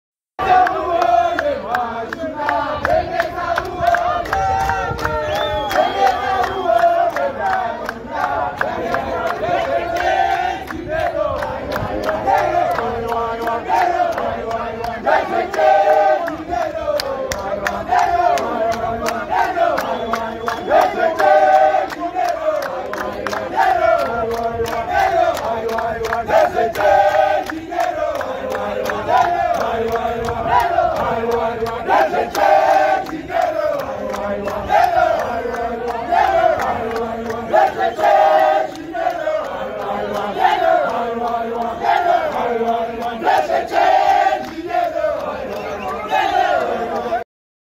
Amanye amalunga ebandla leCCC abebuthane phambi kwedale leHigh Court
Amalunga ebandla leCCC abuthane edale leHigh Court ngesikhathi kuthonisiswa udaba oluphanyekwe yileli bandla olokuxotshwa kwamalunga alo ePhalamende.